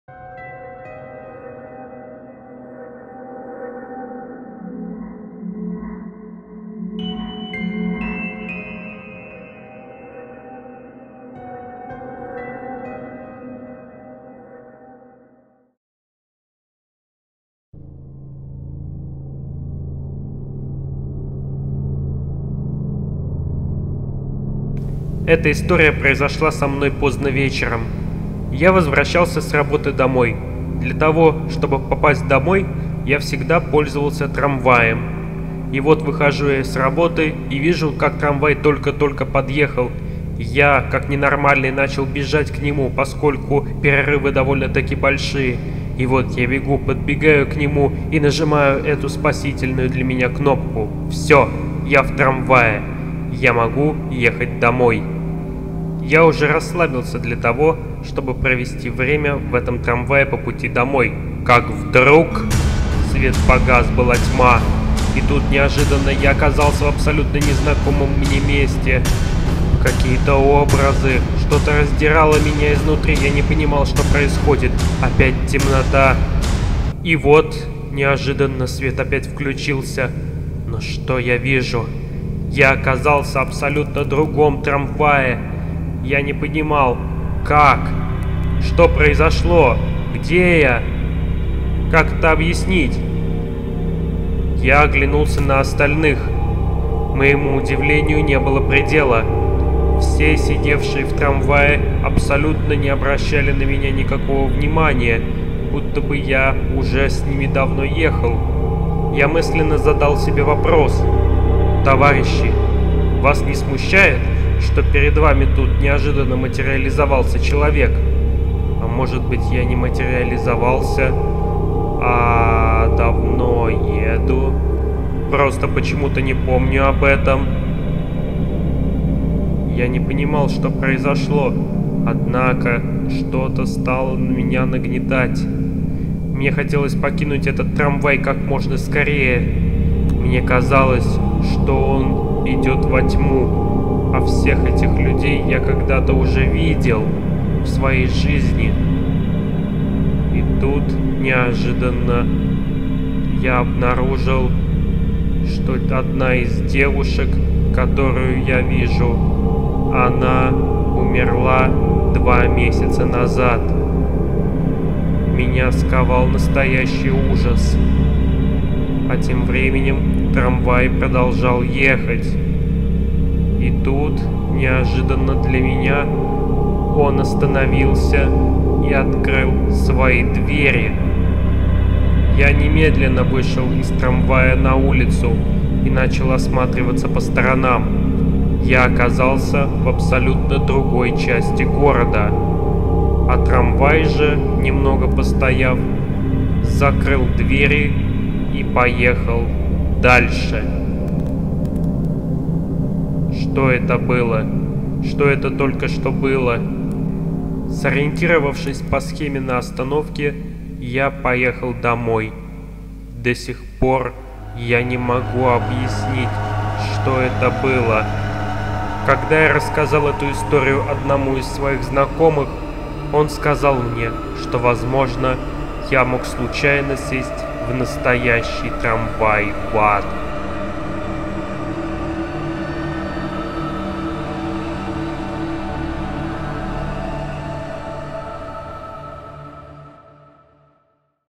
Трамвай / Tram (2018)
Сюжет: Будапешт. Молодой человек после работы садится на трамвай, который увозит его в бездну.

В ролях: Сергей А., рандомные венгры

Режиссёр, сценарист и т.д.: Сергей А.

Страна: Россия, Венгрия
Жанр: Триллер, Ужасы

Снято на Будапештском трамвае.